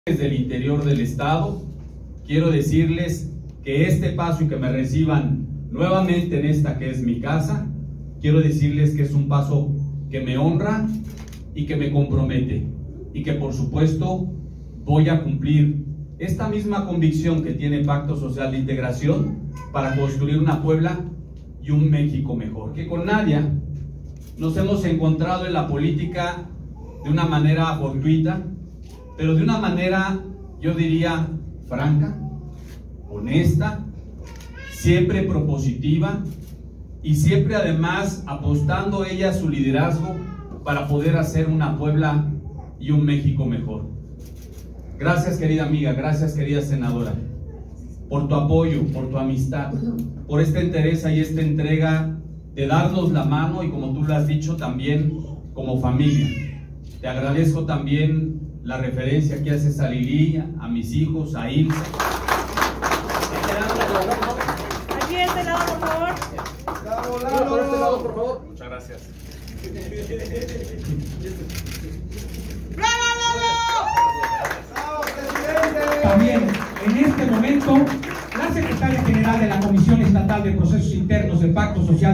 Eduardo Rivera hace su registro como precandidato de PSI a la gubernatura de Puebla
Eduardo Rivera hace su registro como precandidato de Pacto Social de Integración a la gubernatura de Puebla, acompañado del dirigente estatal del PSI Carlos Navarro y la senadora Nadia Navarro